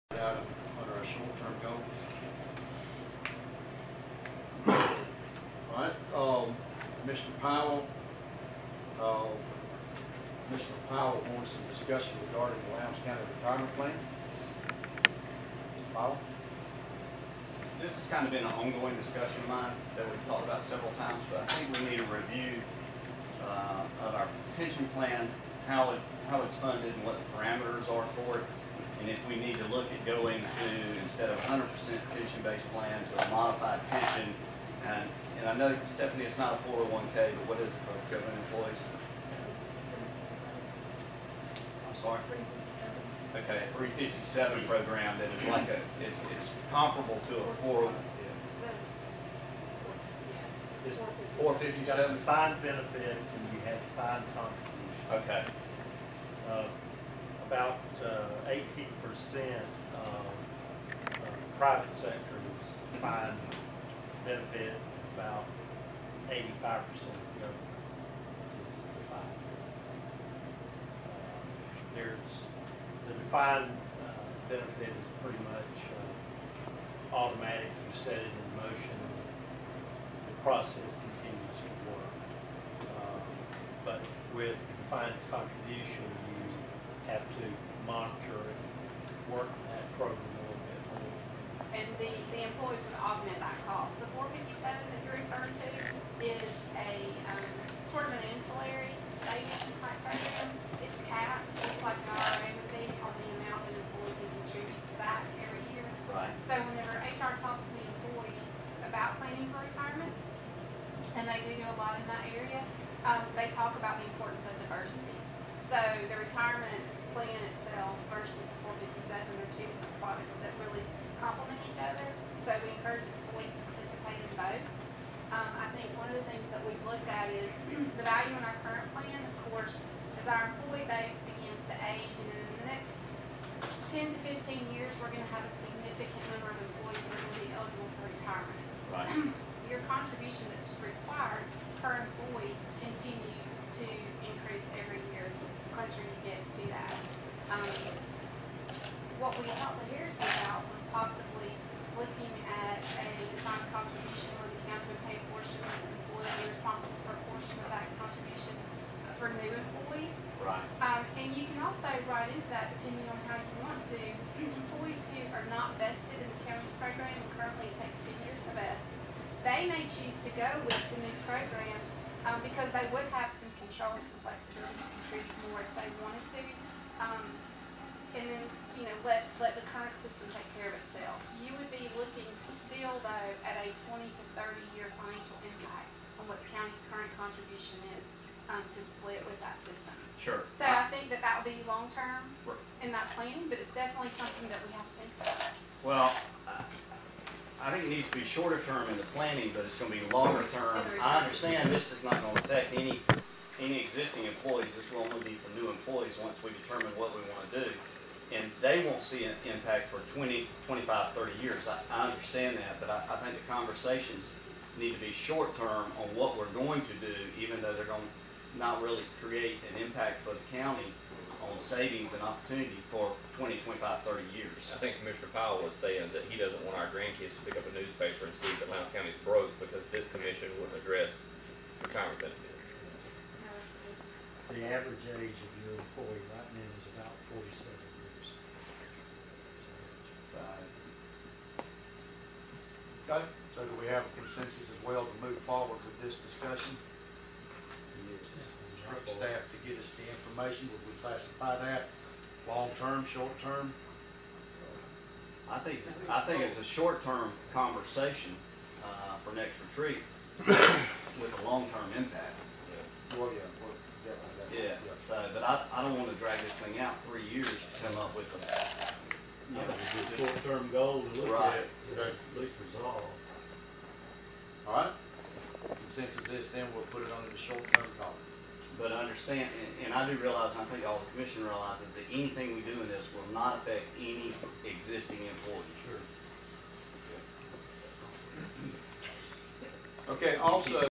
04185
Lowndes County Board of Commissioners (LCC) Retreat Work Session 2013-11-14